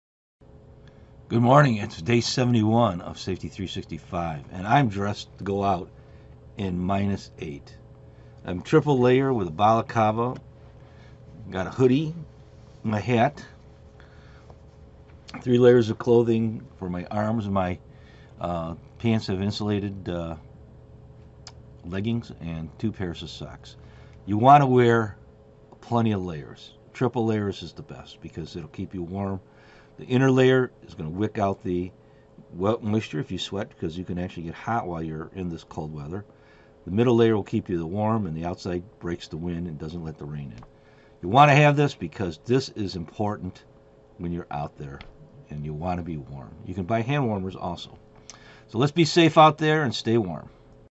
day 71 Safety 365 cold weather
Dress in 3 layer
The first layer should material that wicks away the moisture such as synthetic fibers
The middle layer is the warming layer.
The outside layer should prevent water and wind from coming in.